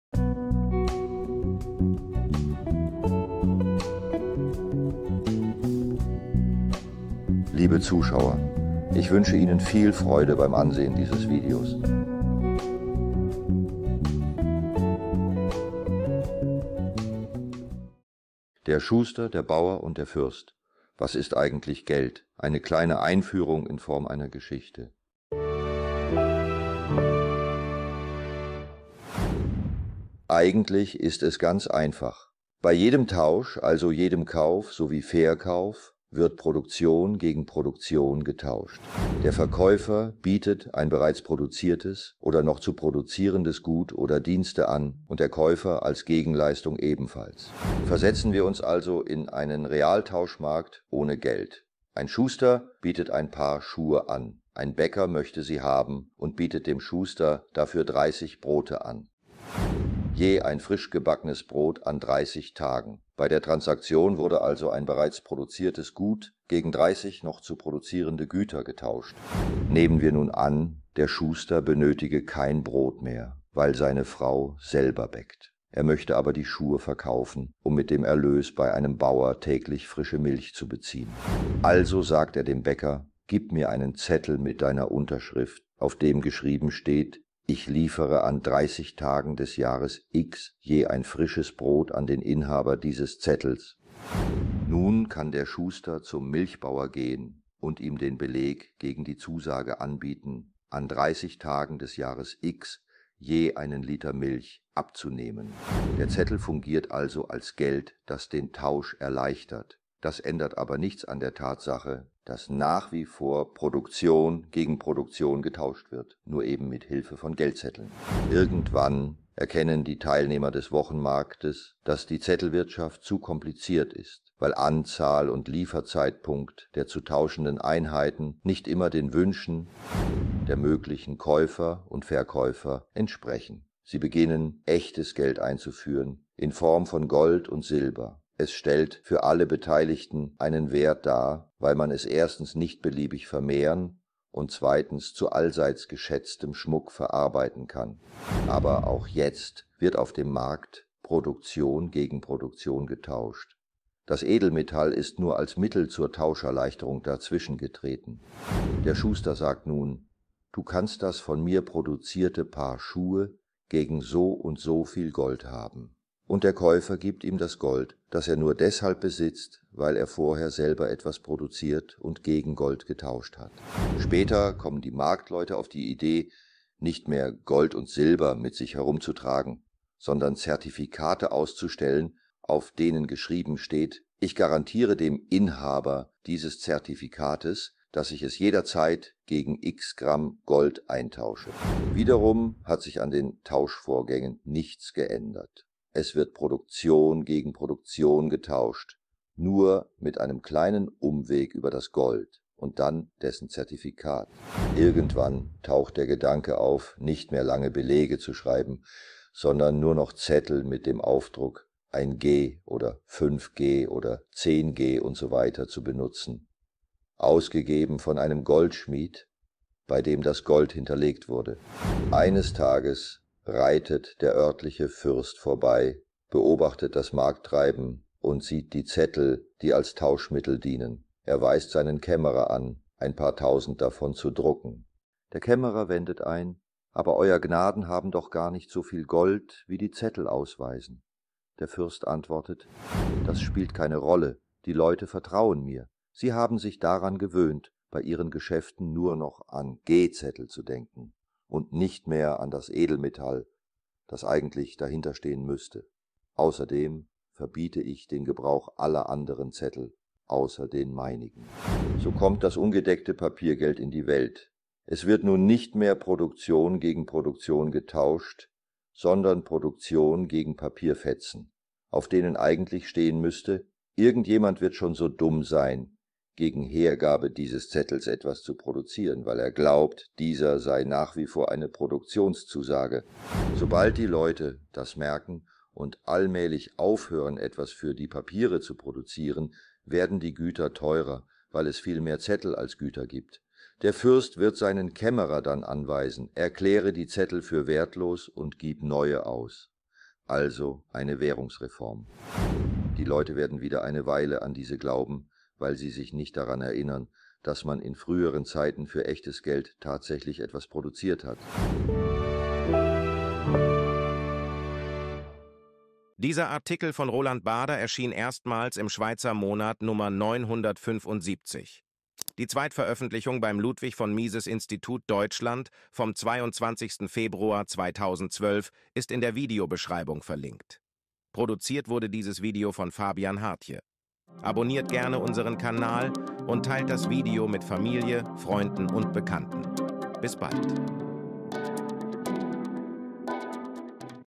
Roland Baader: Der Schuster, der Bauer und der Fürst
Eine kurze Geschichte des Geldes! Warum fingen die Menschen an, Geld zu verwenden? Und was nutzten sie als Geld? Und warum gibt es heute Papiergeld? Roland Baader liefert in diesem Artikel die kurze Geschichte des Geldes! Kurz, präzise und anschaulich.

❓Ist das die echte Stimme von Roland Baader?
Die hier verwendete Stimme ist per KI erzeugt und darf mit freundlicher Genehmigung der Familie Baader von uns verwendet werden. Wir bedanken uns ausdrücklich dafür!

◽ Autoren-Wirken von Benjamin Mudlack: ◽